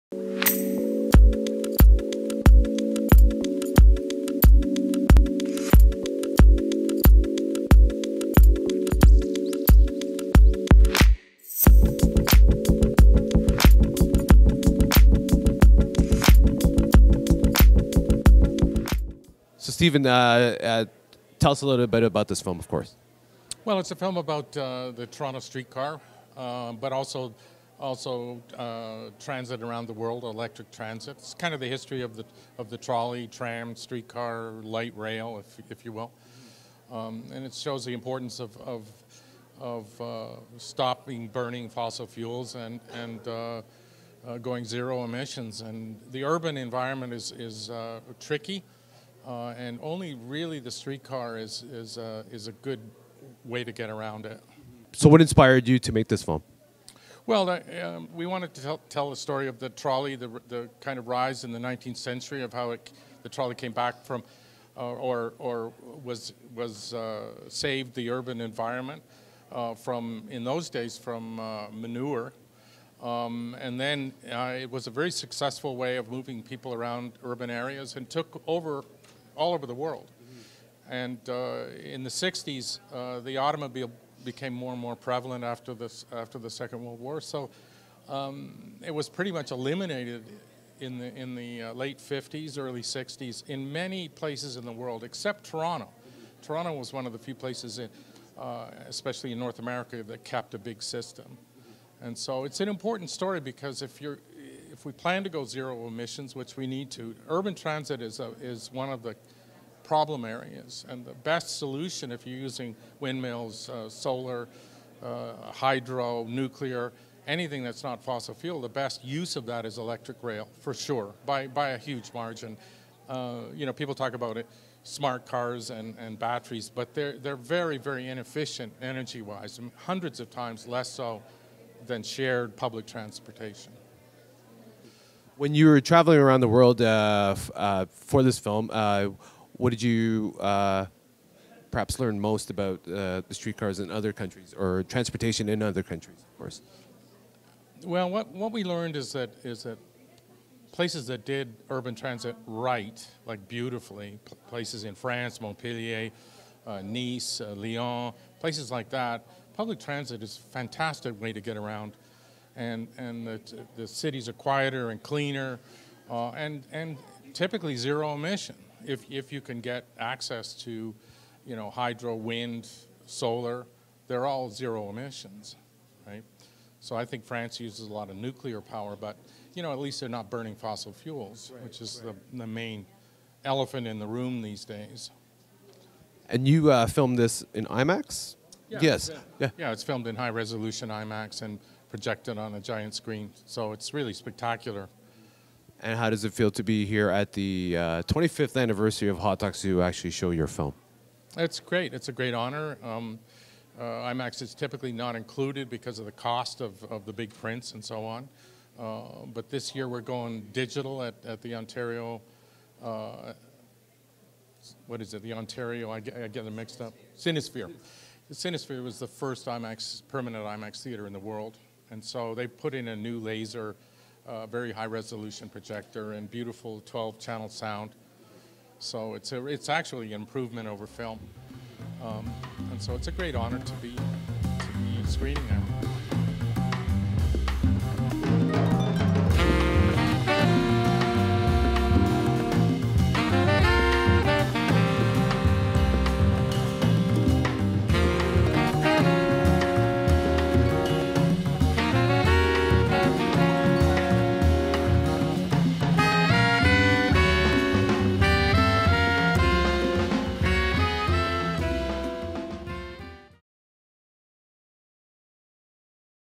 FERNTV Interview with Stephen Low @HOT DOCS 2018
FERNTV interview with director Stephen Low as he discusses his film The Trolley which will premiere in IMAX at the Ontario Cinesphere during the Hot Docs Canadian International Documentary Film Festival.